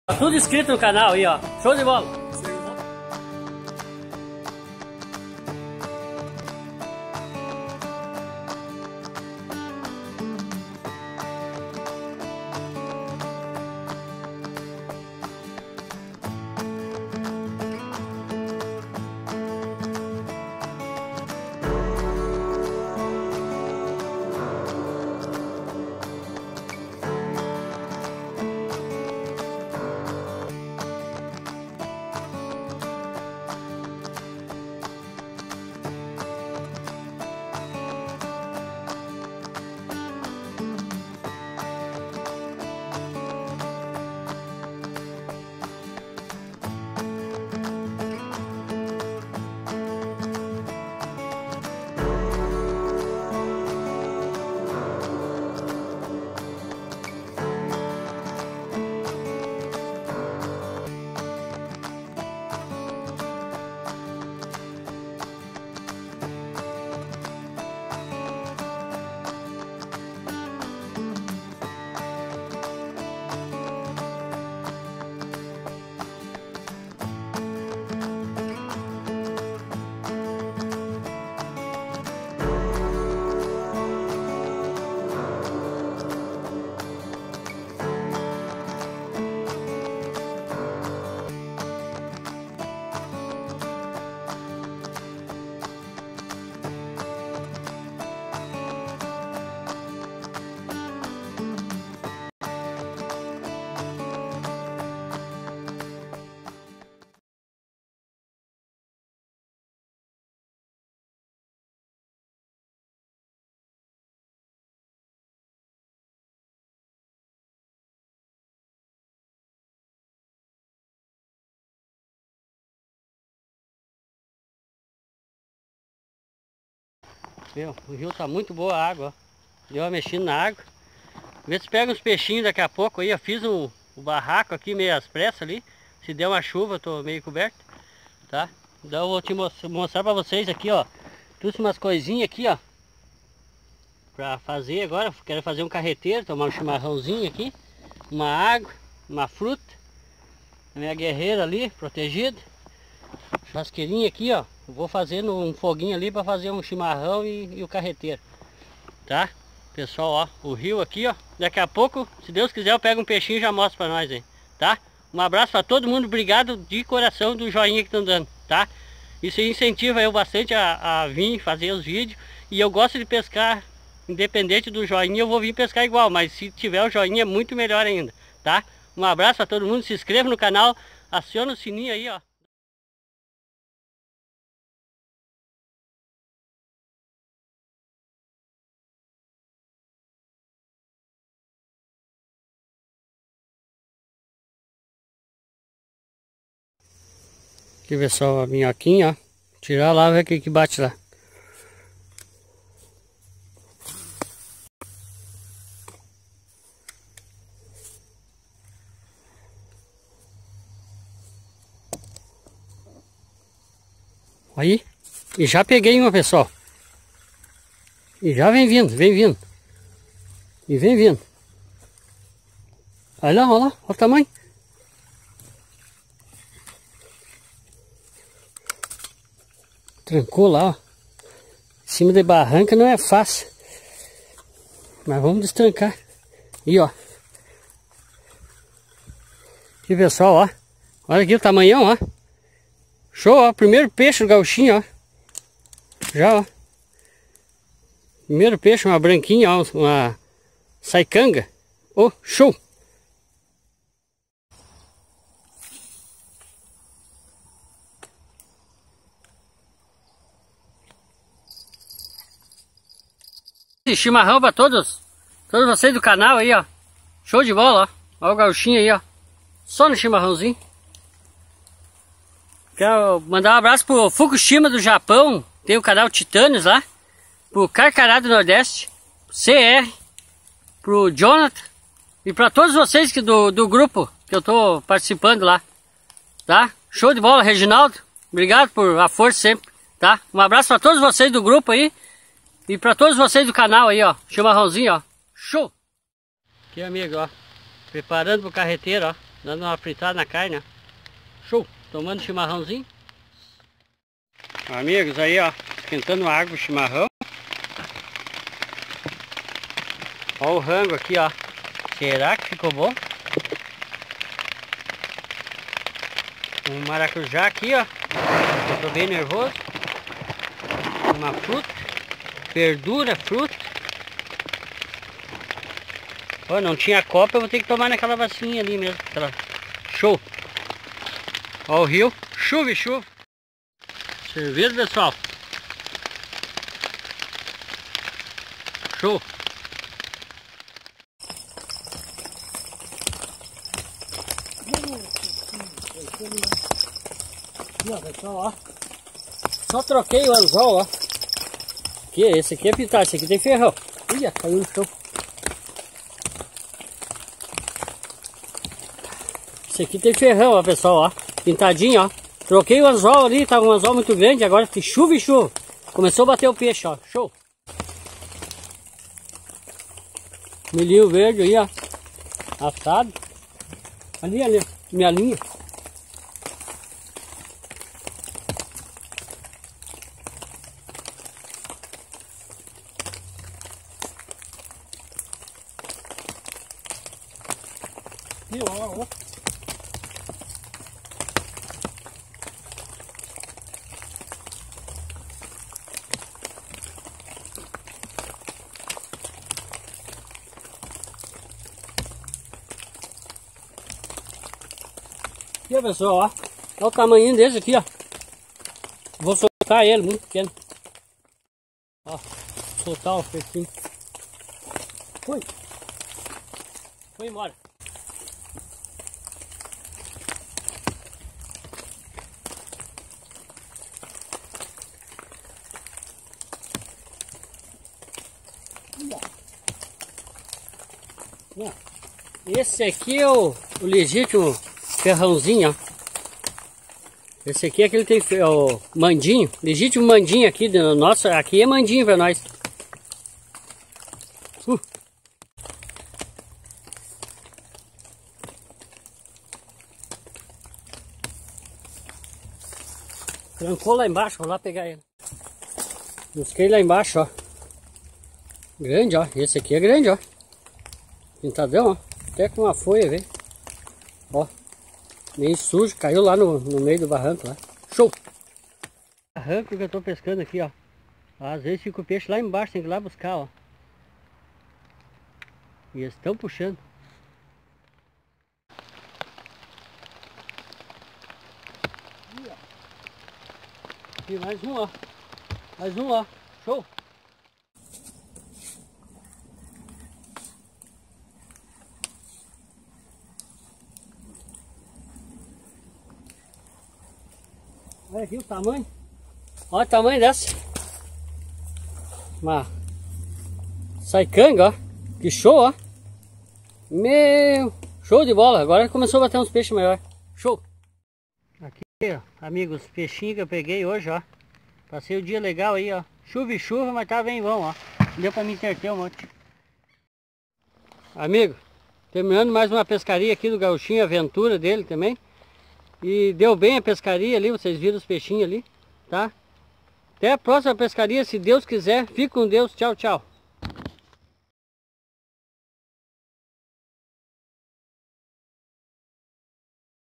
PESCARIA Com CHUVA
Aventura das boas chimarrão comida Boa e uns peixinhos e abraços a todos amigos  gostar da aquele 👍